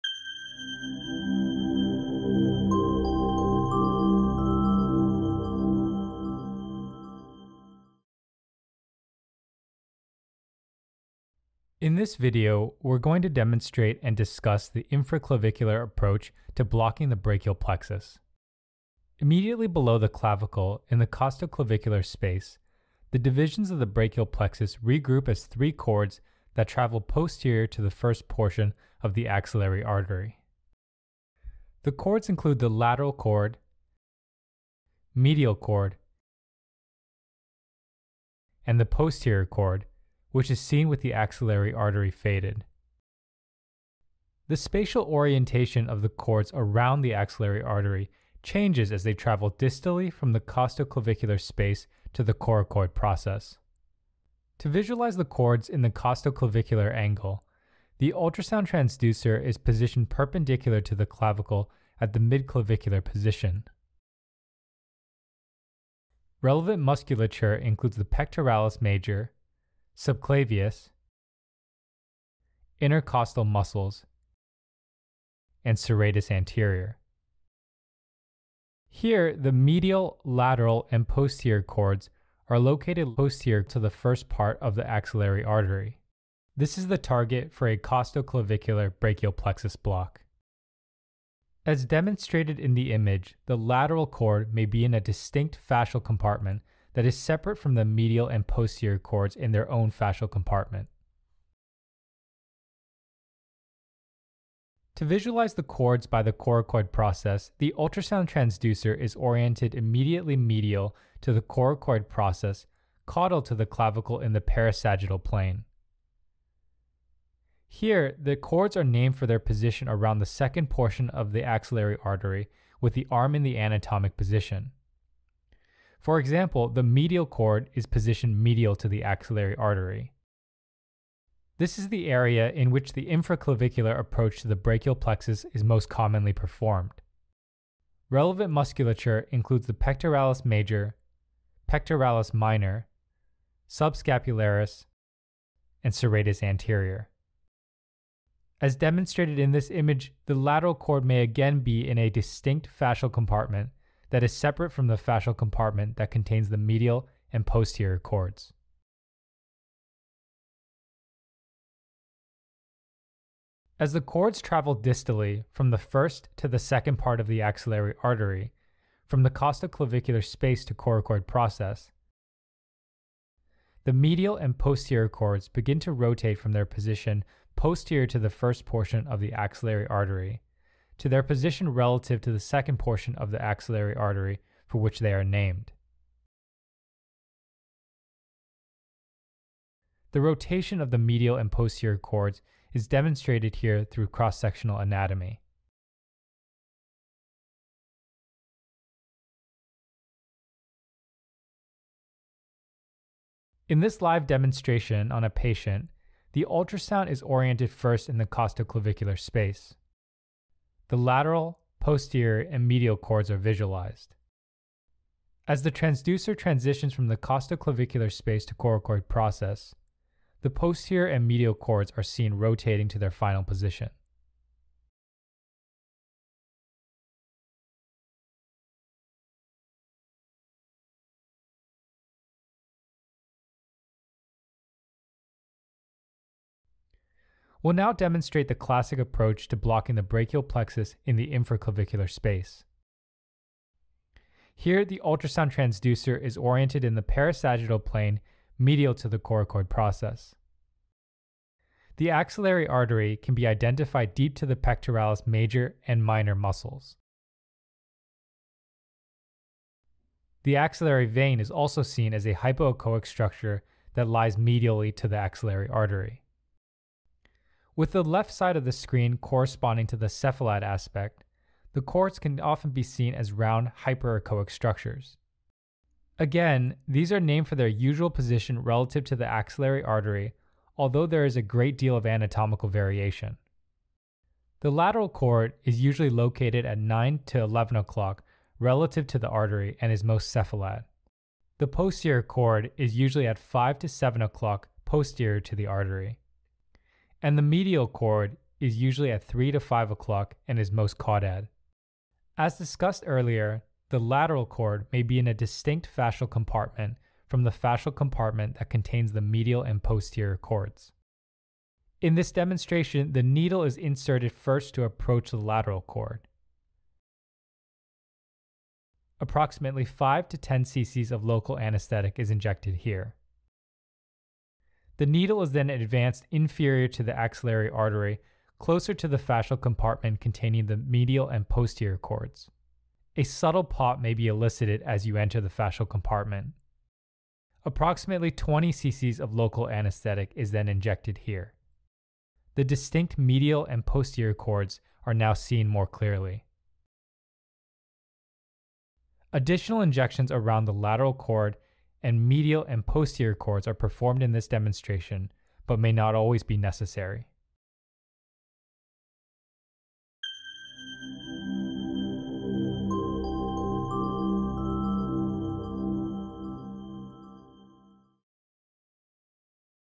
Infraclavicular Nerve Block // Ultrasound-Guided Demonstration
We review the basics of an ultrasound-guided infraclavicular nerve block including relevant anatomy and a patient demonstration. Ultrasound orientation may vary by institution.

Procedure performed by Charles Kelly, MD. Peer reviewed by Raf Vazquez, MD.

Cross-sectional images and additional graphic arts support by Body Scientific International.

01:05 Anatomy at costoclavicular space
02:02 Anatomy at coracoid
03:09 Transition from costoclavicular to coracoid
03:58 Patient demonstration
Equipment:
Sonosite PX
StimuQuik Insulated Peripheral Nerve Block Needle 90 mm 21 Ga.